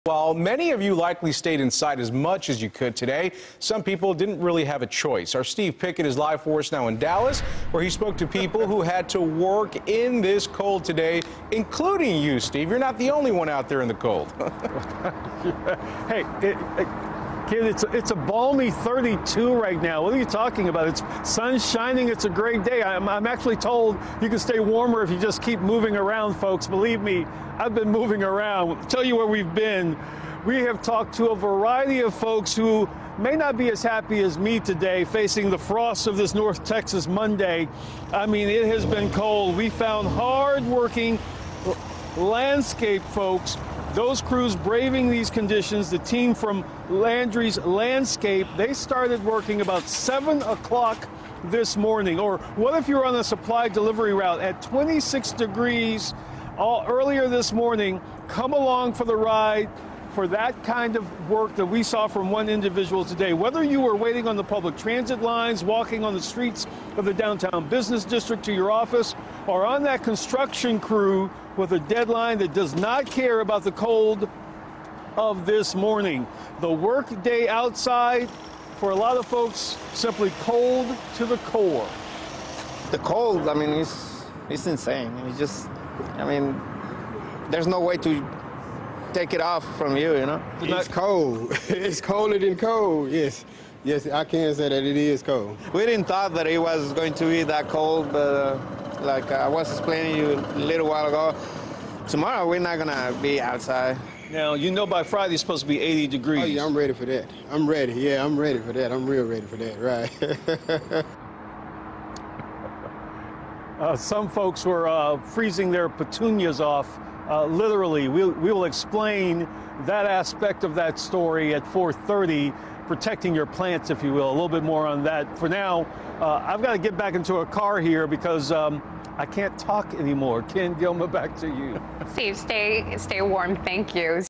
Tough Stuff Working Outside During Hard Freeze
CBS 11's Steve Pickett talked to folks, like himself, who had to be outside with temps in the mid 20s.